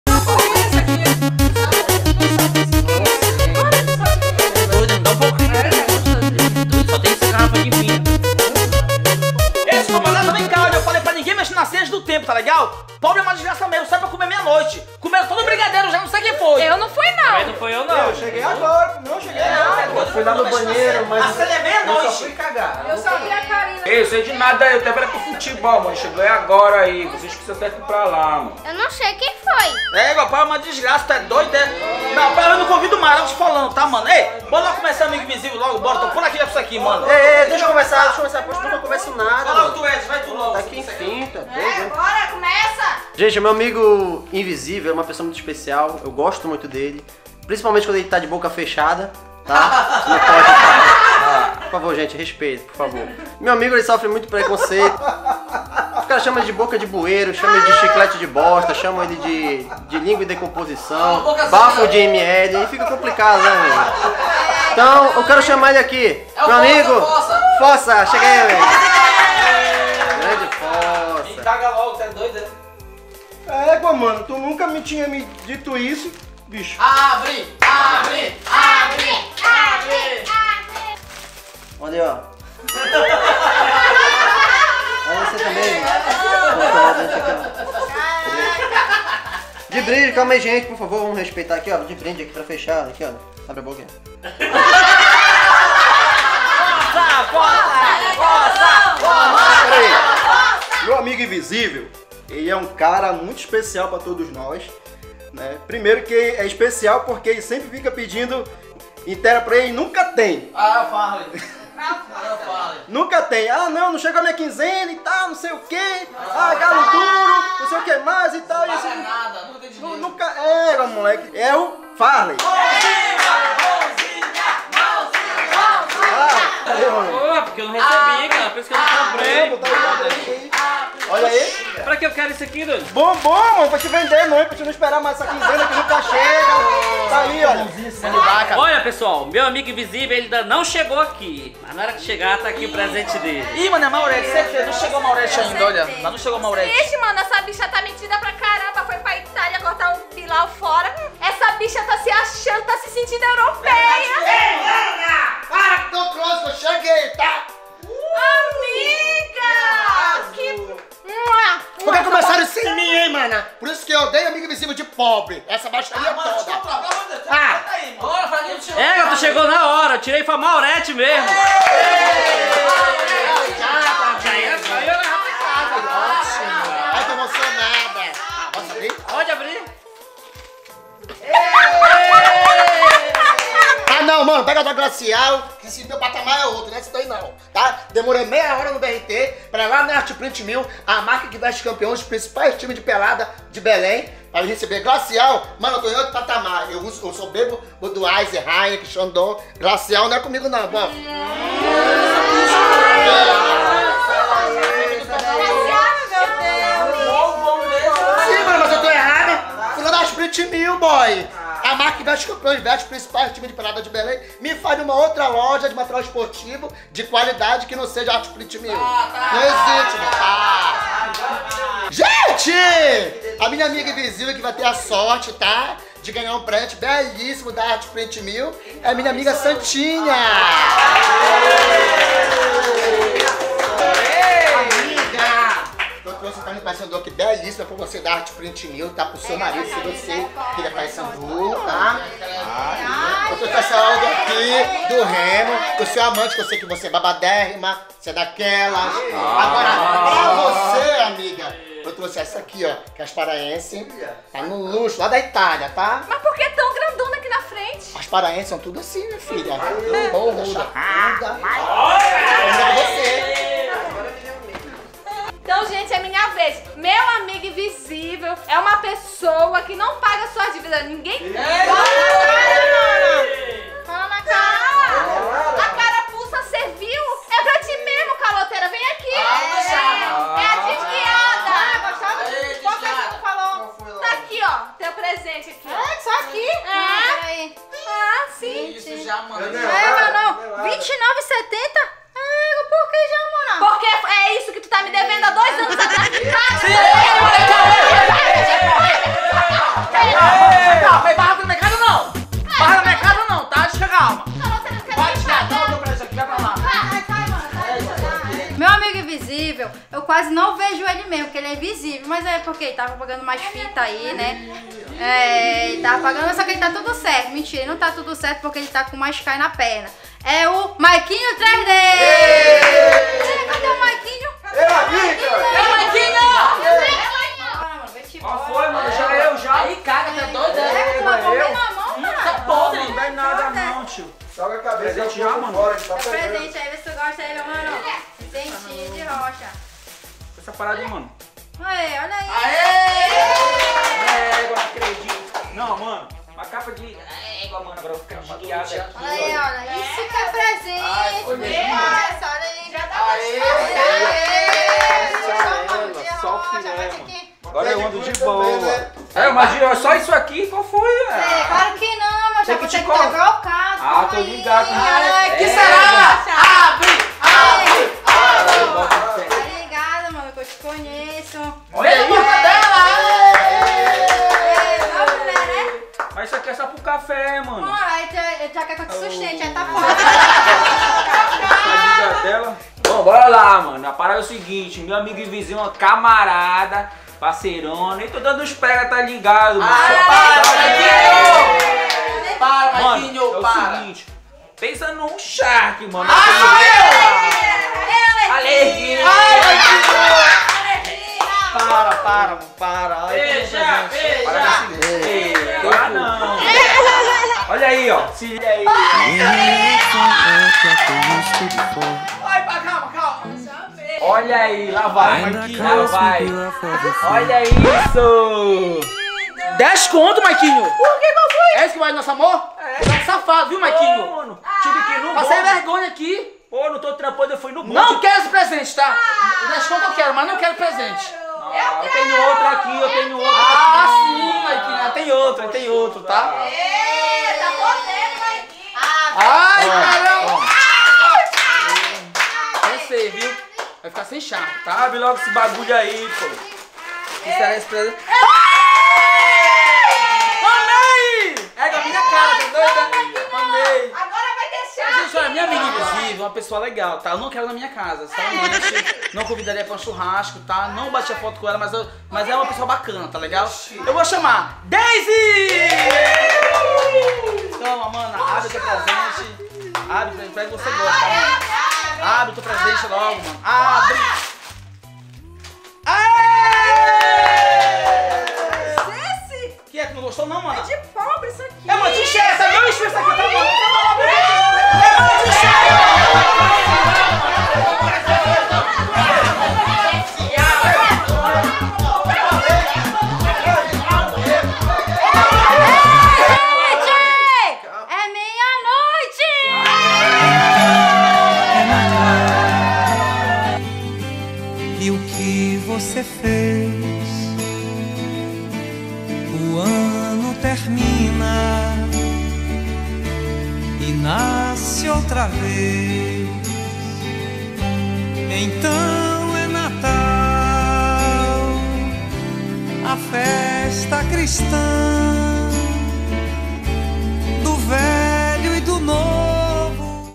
AMIGO SECRETO DE POBRE
Bob Fllay e sua equipe PANELINHA2.0 Mostram como é o amigo invisível dos paraenses, SUCURITUBE, FARLEY VIDAL, SANTINHA, ESDRAS AMORIM no melhor vídeo de natal de 2020